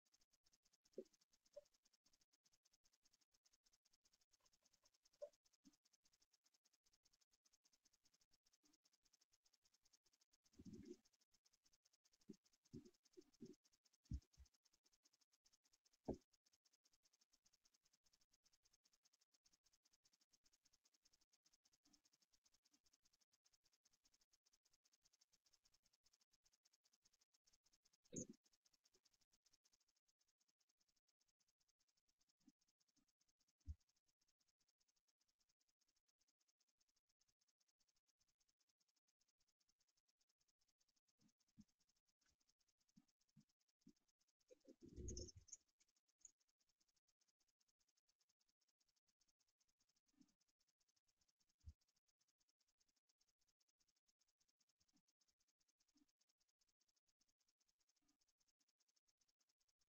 Rennmäuse
Cody wirft sich auf den Rücken